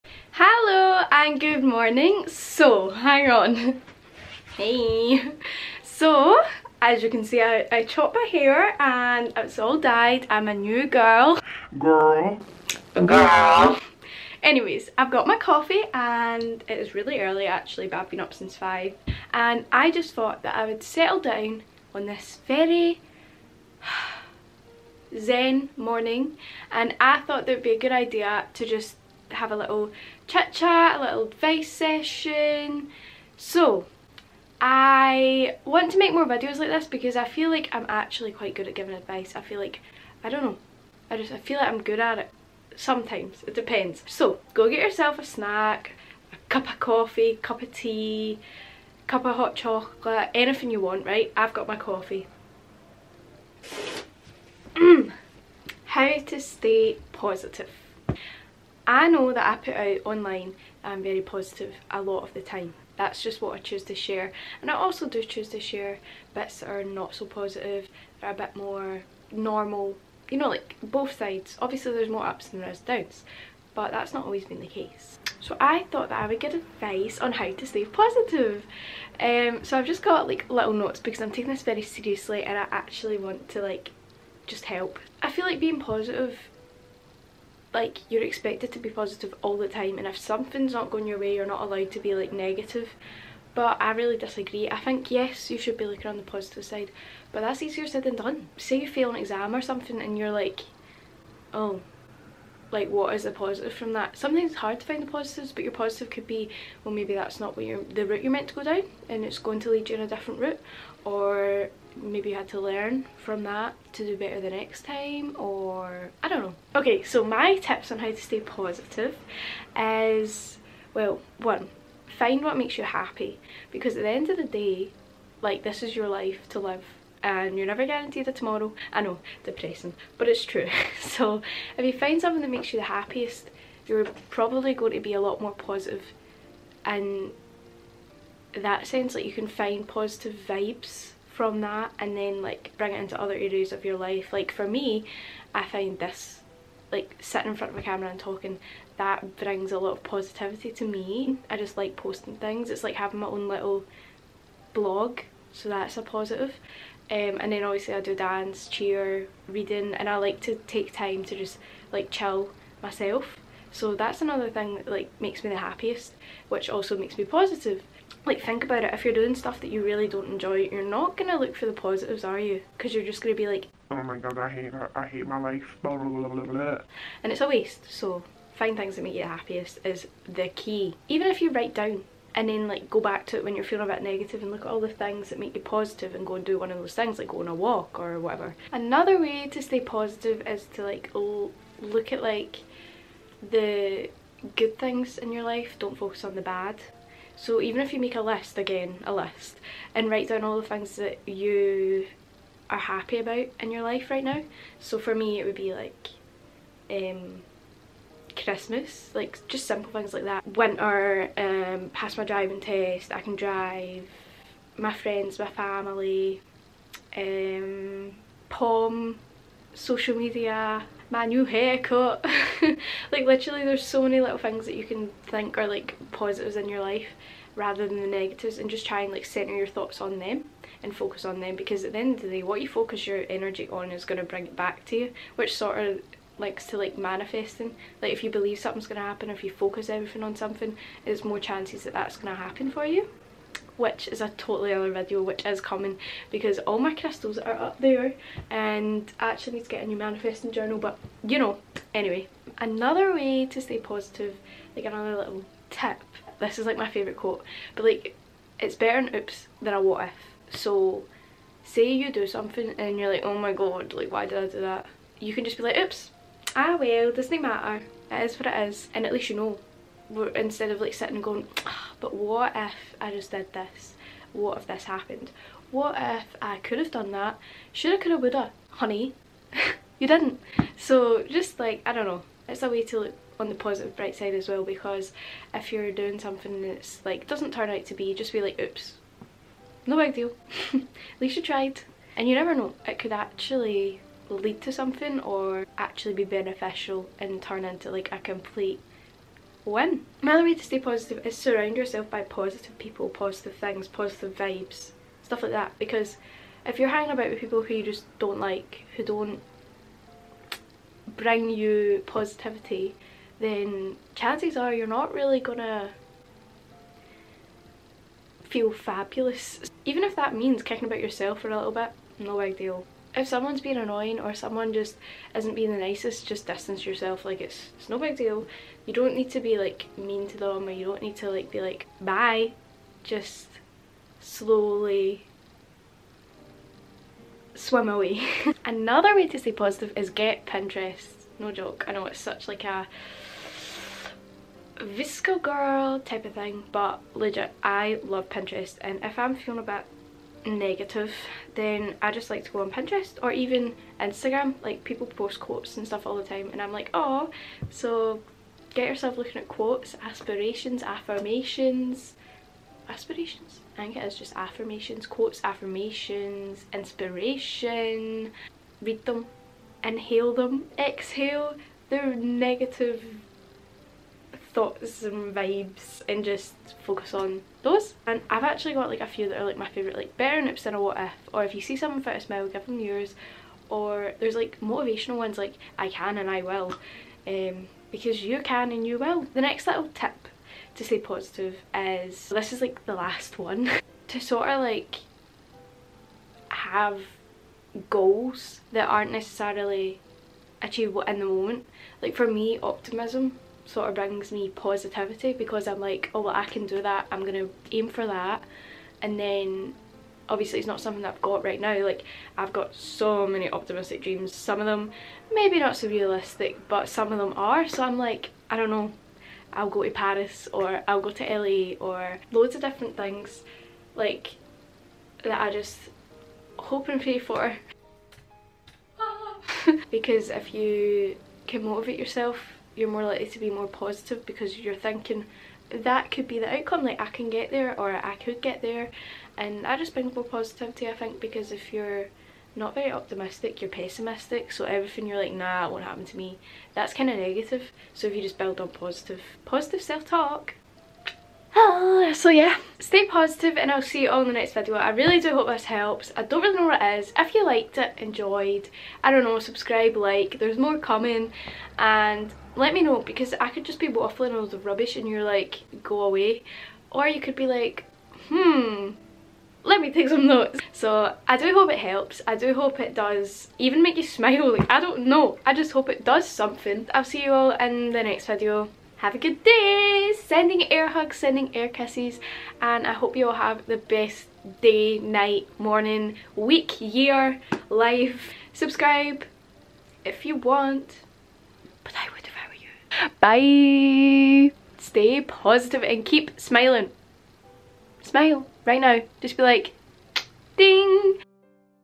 how to stay positive - life advice part 1
Hello! I AM SO GLAD YOU FOUND THIS.
I really want to use my voice to help other girls who are struggling :)
I hope this video helps and makes you smile, you ARE amazing!!!

So for episode 1: a little sit down chit chat on how to stay positive is the topic...I truly hope that this brings you some from of help or light.
You can achieve anything, so keep smiling! Settle down with a little cup of tea/coffee and just keep calm!

Sending you so much positivity and air hugs xx
Let's have a good life together!
Lots of Love, wee toni marie xoxox